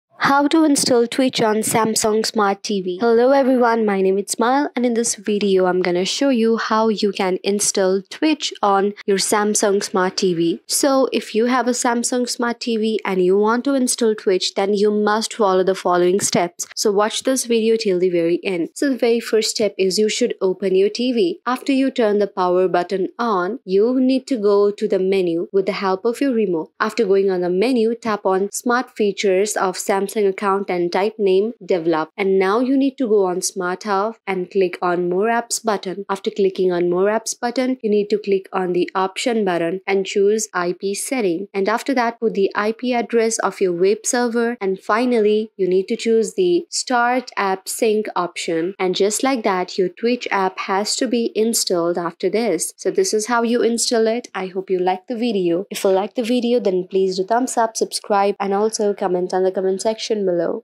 How to Install Twitch on Samsung Smart TV
Hello Guys,

Welcome to Plug Play Pause,

Want to enjoy Twitch on your Samsung Smart TV? In this tutorial, I'll show you how to install and set up Twitch on your Samsung Smart TV.

Whether you're into gaming streams, live events, or creative content, I'll guide you through the steps to get Twitch up and running on your TV screen.